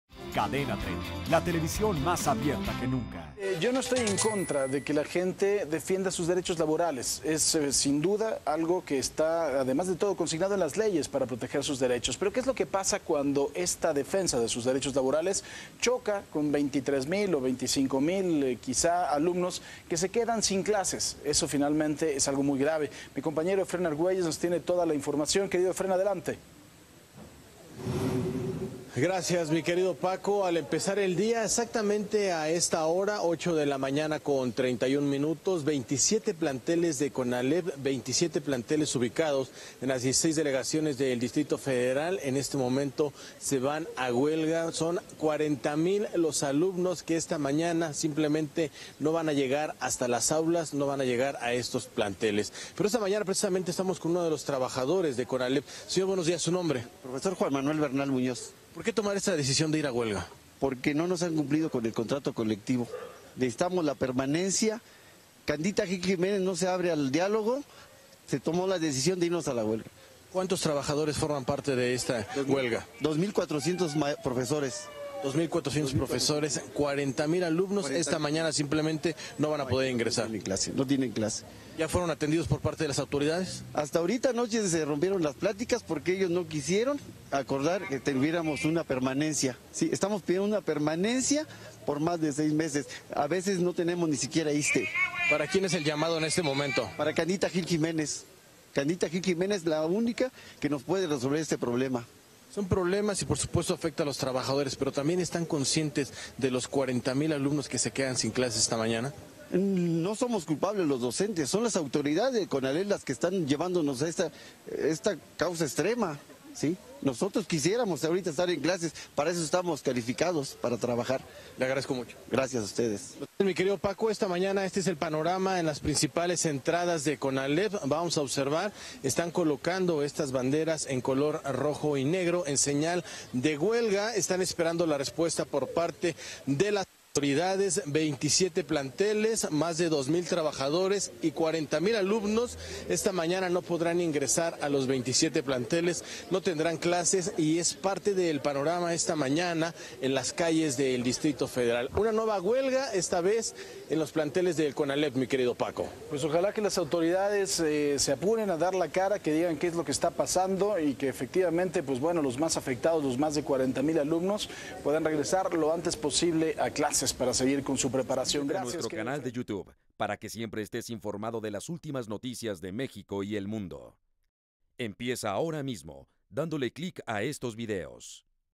Estalla huelga en planteles del Conalep en el Distrito Federal
Estalla huelga en planteles del Conalep en el Distrito Federal. Las banderas rojinegras fueron colocadas en los 27 planteles, por lo que más de 40 mil alumnos se quedaron sin clases.